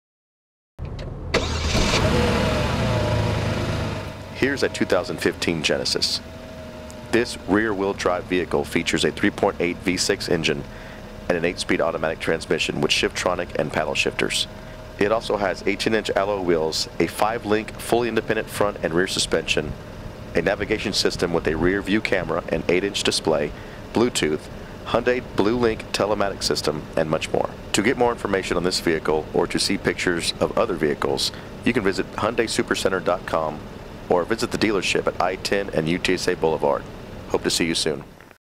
2015 Hyundai Genesis from Red McCombs Supercenter
Here is a short description of a 2015 Hyundai Genesis  from Red McCombs Supercenter in San Antonio, Texas.